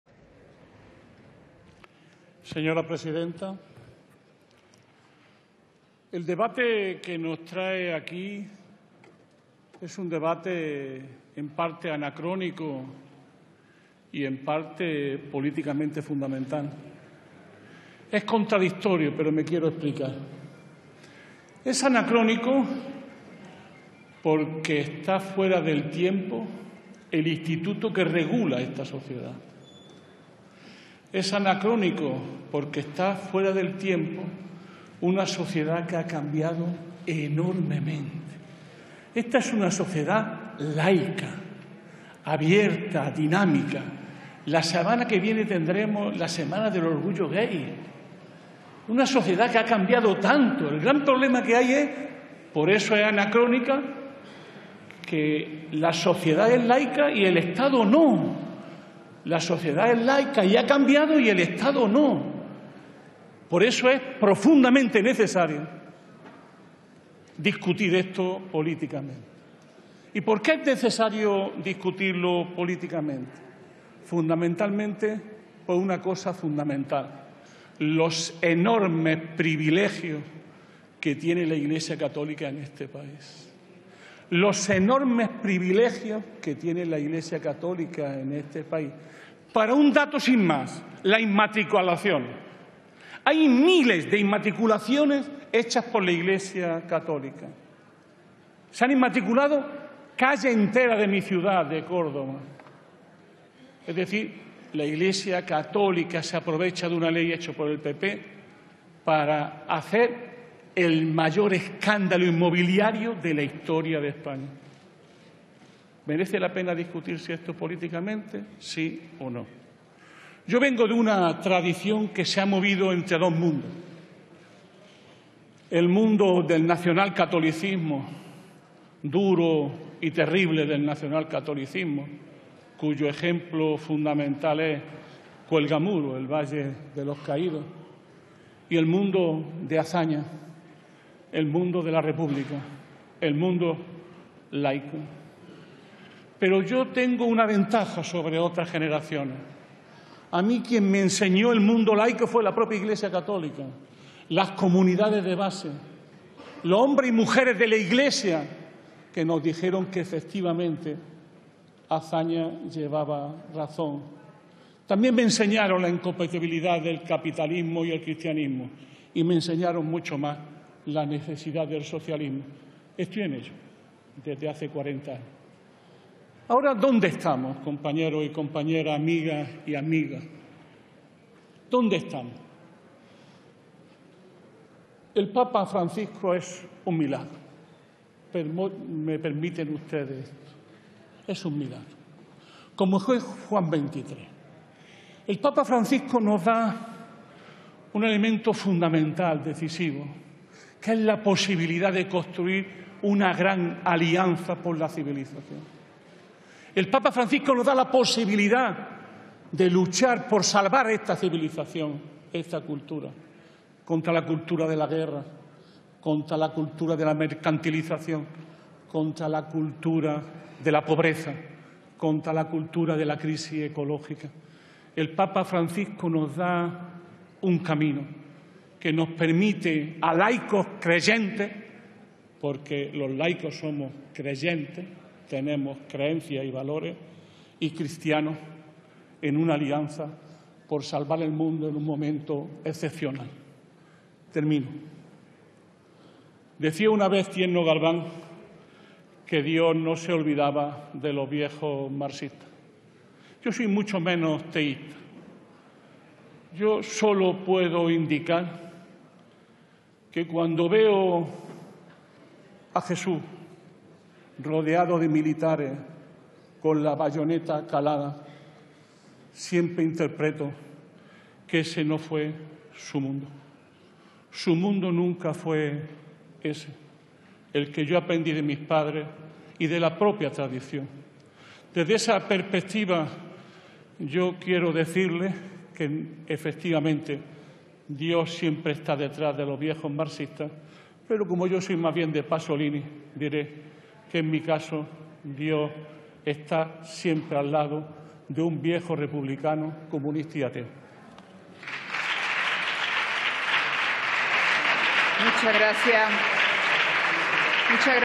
La sociedad es laica y ha cambiado; el Estado no. Por eso es profundamente necesaria esta discusión
Manolo Monereo en el debate de la PNL sobre garantizar la libertad ideológica, religiosa y de culto.